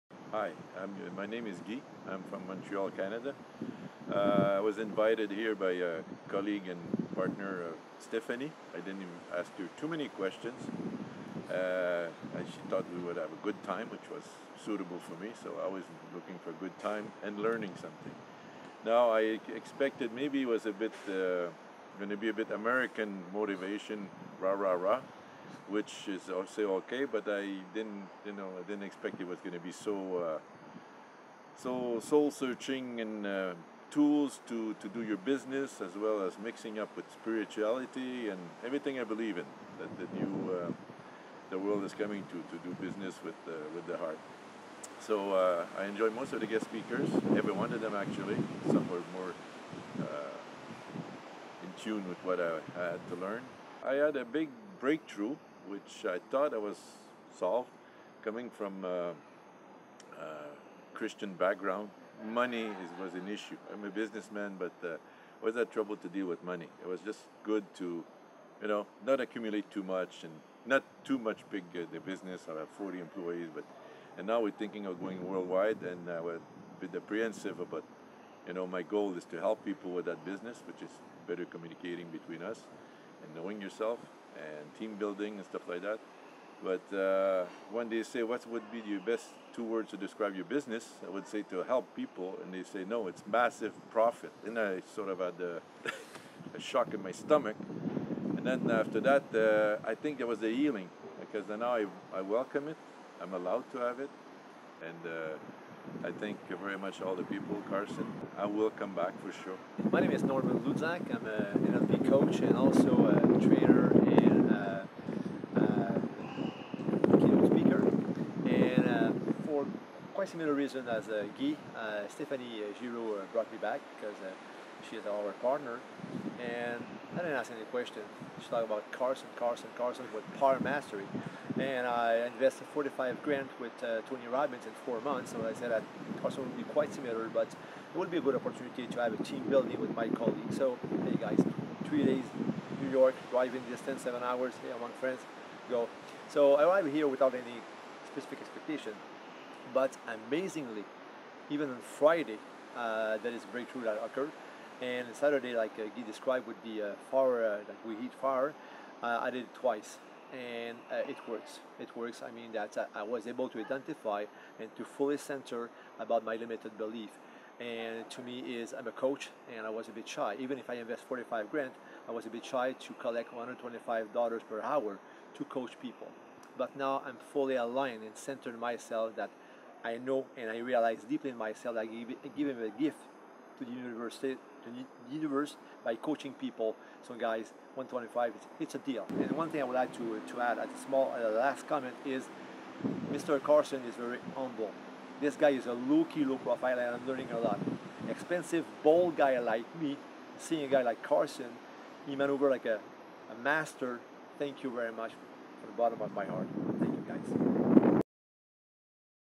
Powermastery Changing Lives, Careers, and Businesses In Just 1 Weekend!
Powermastery was created to provide a conduit to achieving breakthrough for life, career and/or your business from the inside out. We employ fun and memorable hands-on workshops and the power of experience and metaphor to embed, design and shape a new empowering behavior and belief system over stale dysfunctional old believes.

It is a ground breaking and results driven platform for positive change.